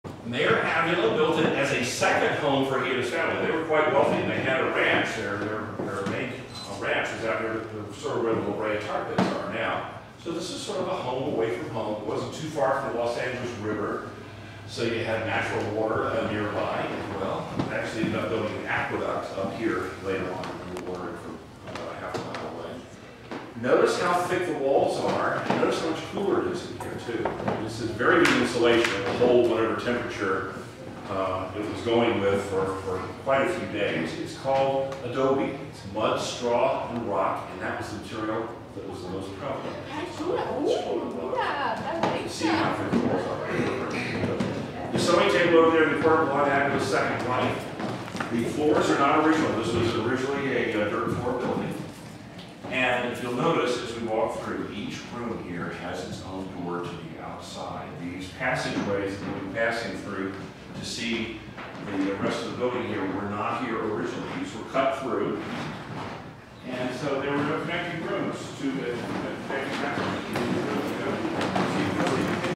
Tour of LA by bus Part 6
Bob our tour guide telling us about an old Mexican house. Shot on 17th February 2011.